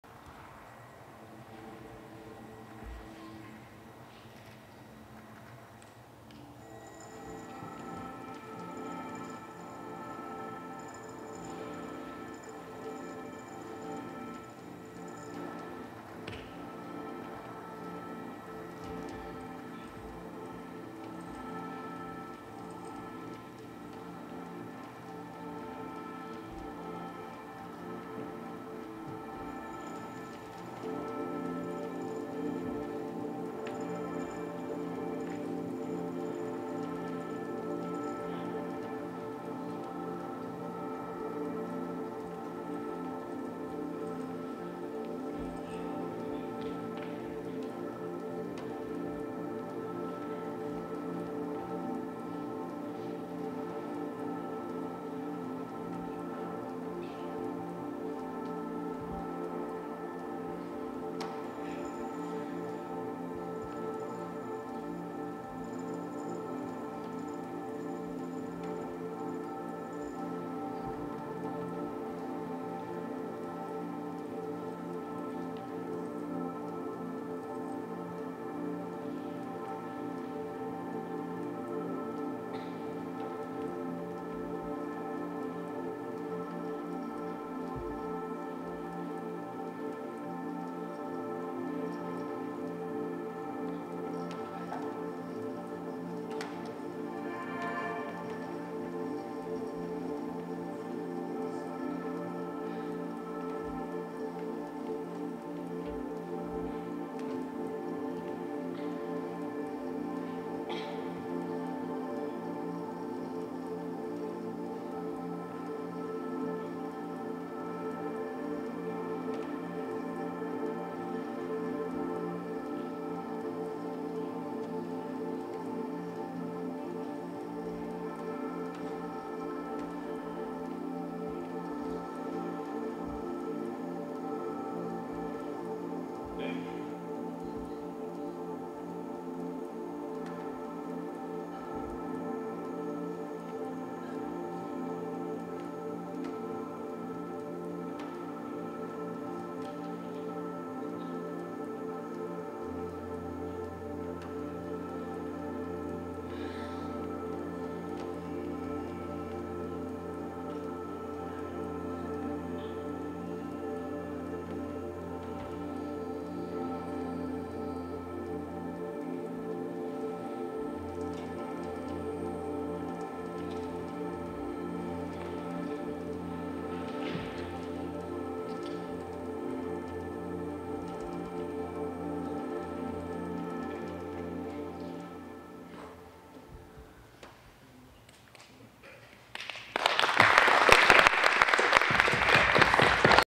Master Minds 28 april kl.12.30.MOV
Dansföreställningen Master Minds 28 april 2011 kl.12.30 på Martinssonhallen i Bygdeå/Robertsfors.